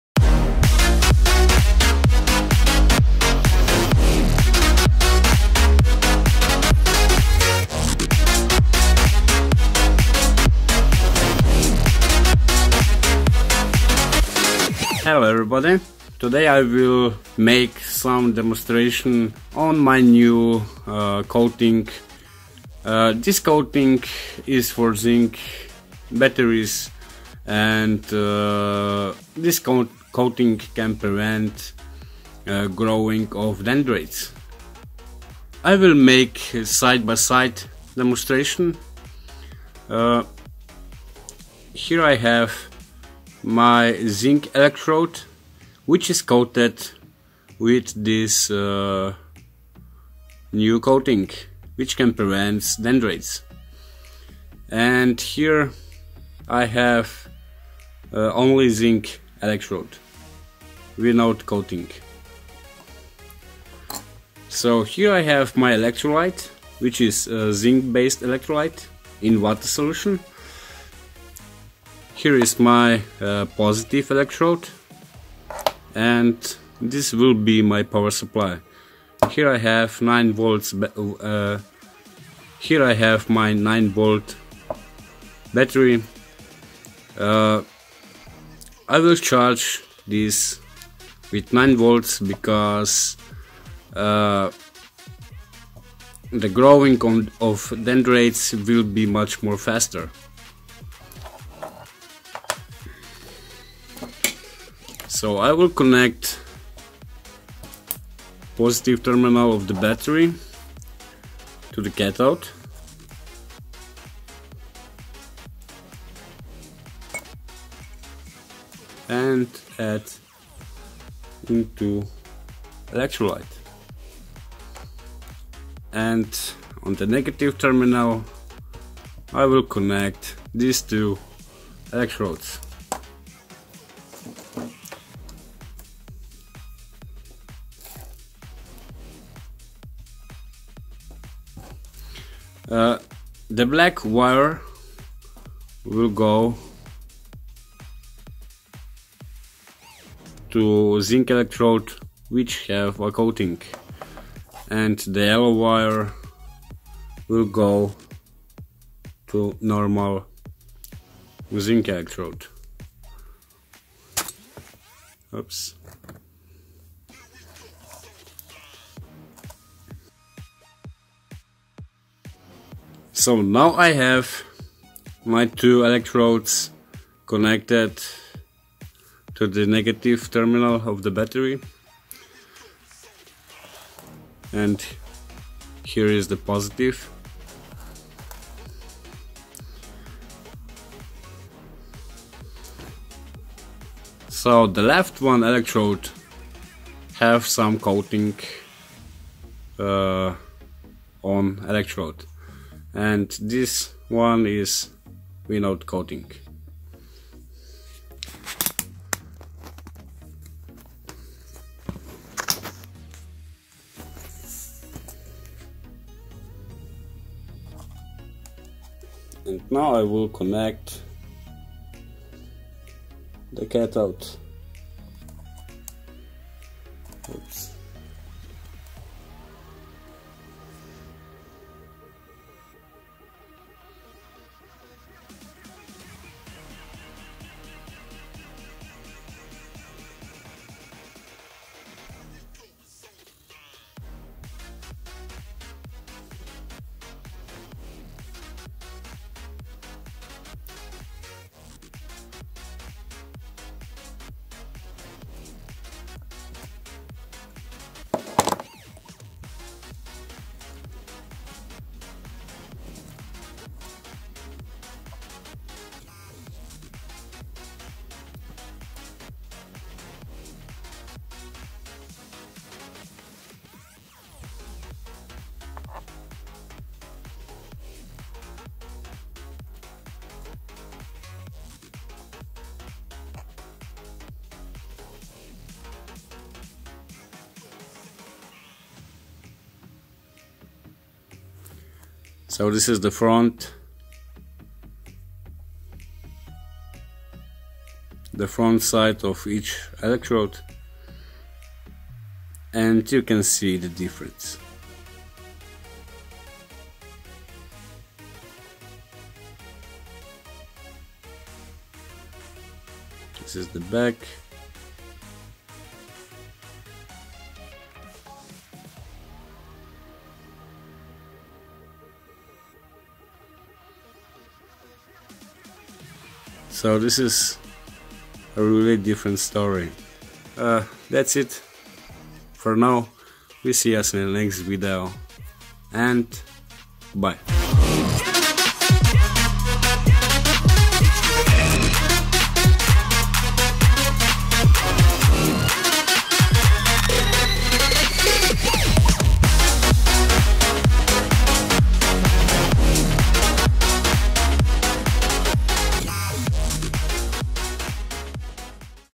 Rechargeable Zinc battery - dendrites update
In video "Rechargeable Zinc battery - dendrites update" I try to eliminate the growing of dendrites which are the problems in rechargeable zinc batteries.
I am on a really good way because I reduce the groving of the dendrites from 98 to 95%

or